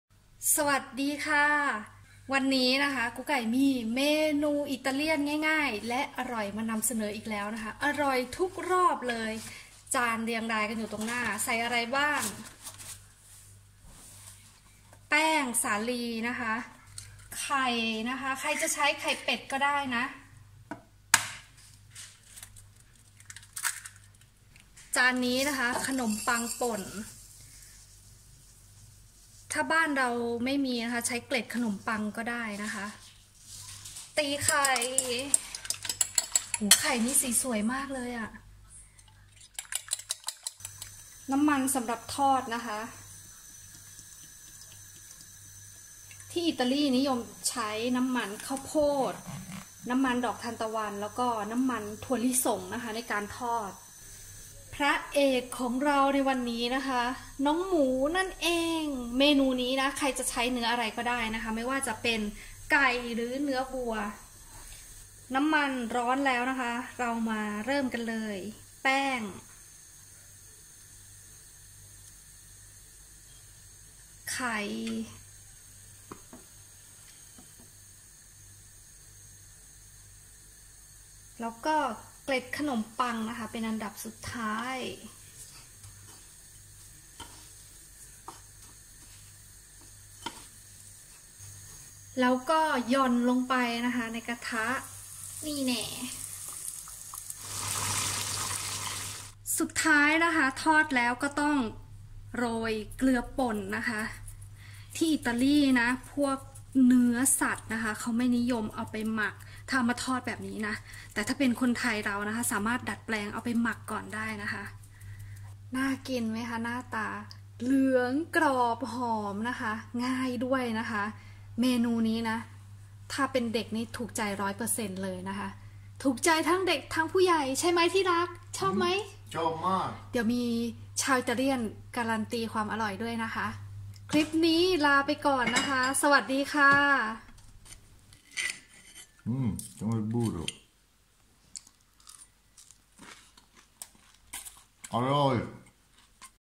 แค่ส่วนผสมไม่กี่อย่าง หมูทอดนุ่มละลายในปาก​ Italian​food​
for inglish subtitles press CC อาหาร​อิตาเลี่ยน​ หมู​ทอดสไตล์​อิตาเลี่ยน​ หมู​ทอด​นุ่ม​ละ​ลา​ยใน​ปาก​